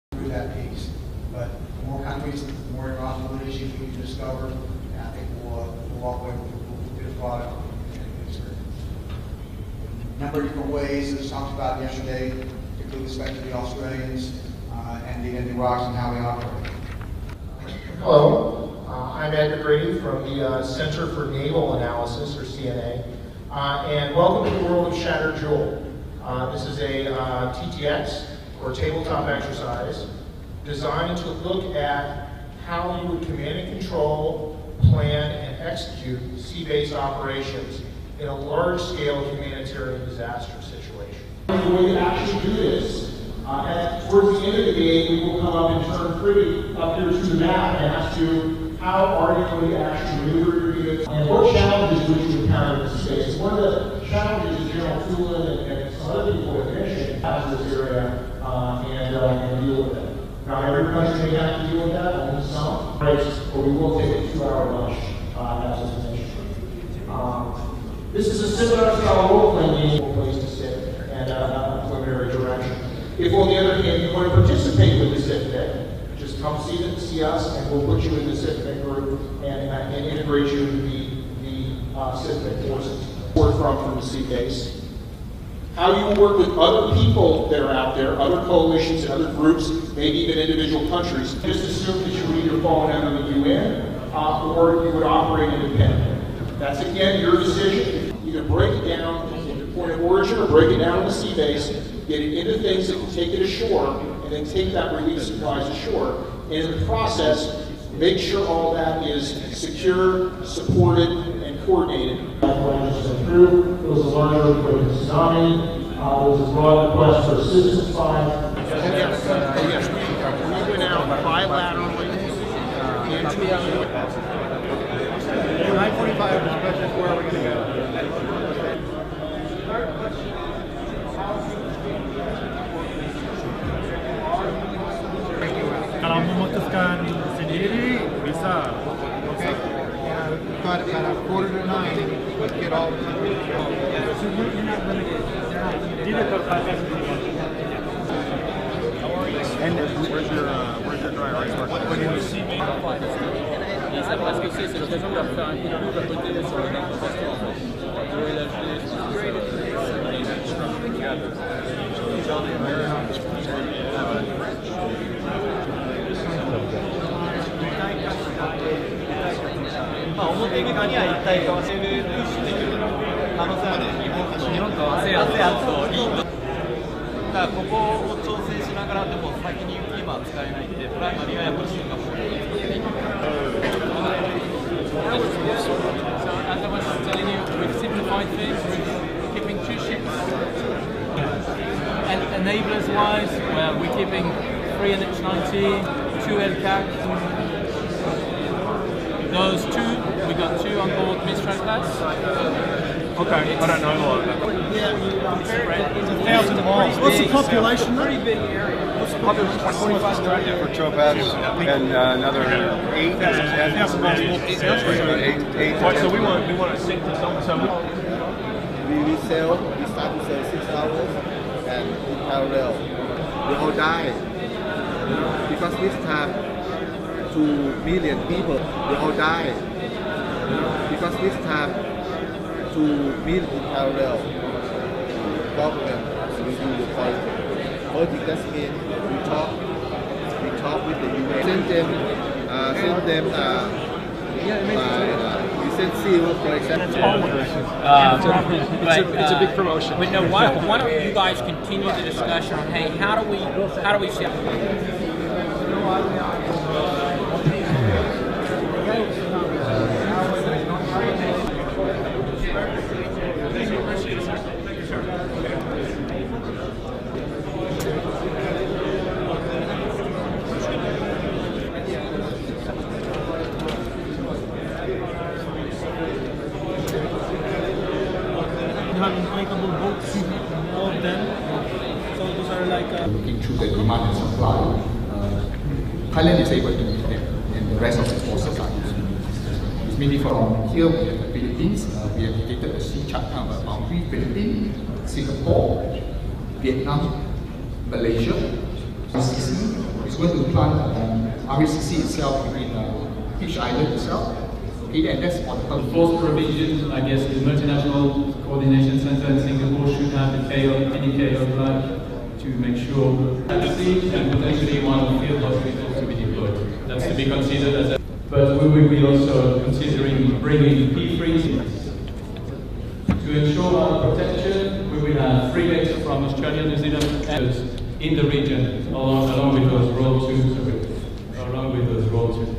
160712-M-SE916-001
U.S. and foreign military leaders participate in a seabasing table-top-exercise during USPACOM Amphibious Leaders Symposium (PALS) in San Diego, Calif., July 12, 2016. PALS brings together senior leaders of allied and partner nations from the Indo-Asia-Pacific region to discuss key aspects of maritime/amphibious operations, capability development, crisis response, and interoperability. Twenty-two countries, including the U.S., are participating. (U.S. Marine Corps video by Sgt. David J. Adams/Released) [779RS90_NMNXIYBF6_fc.mov]